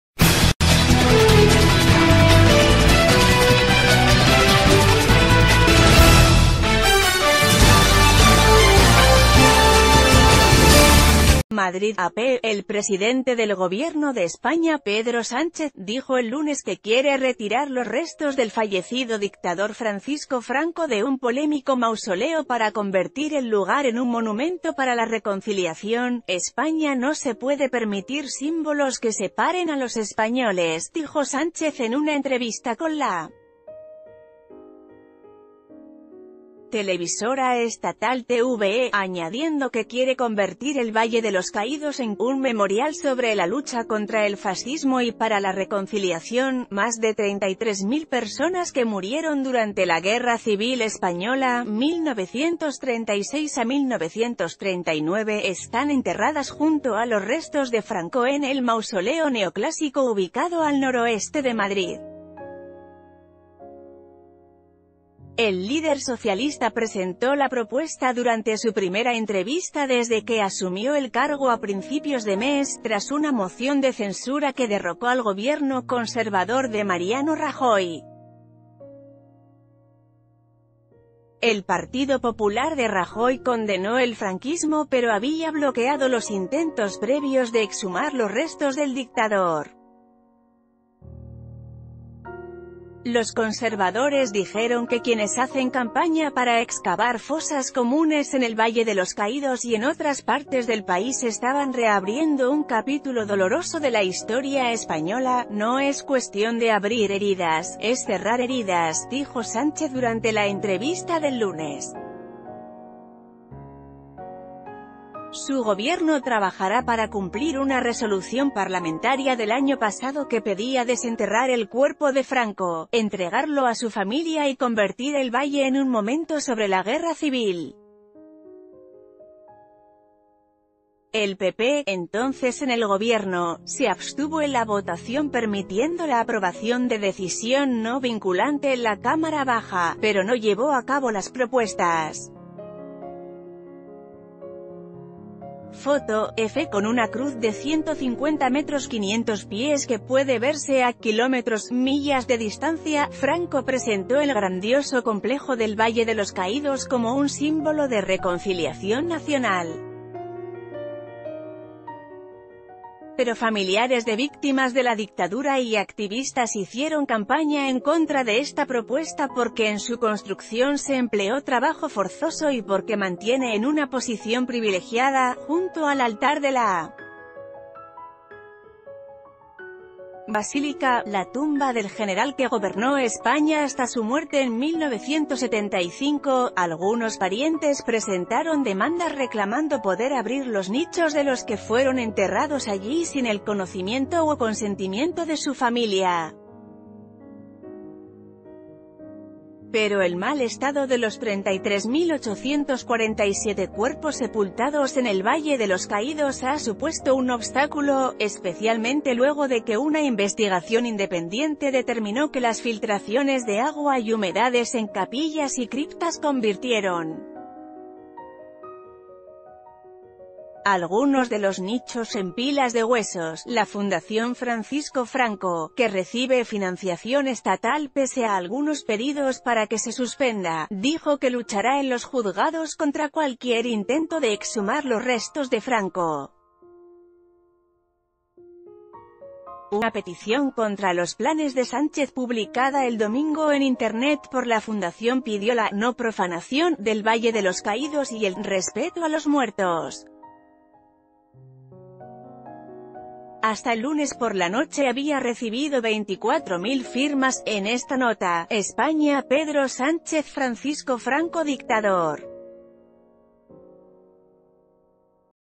España: Sánchez promete exhumar restos de dictador Franco
MADRID (AP) — El presidente del gobierno de España, Pedro Sánchez, dijo el lunes que quiere retirar los restos del fallecido dictador Francisco Franco de un polémico mausoleo para convertir el lugar en un monumento para la reconciliación.“España no se puede permitir símbolos que separen a los españo...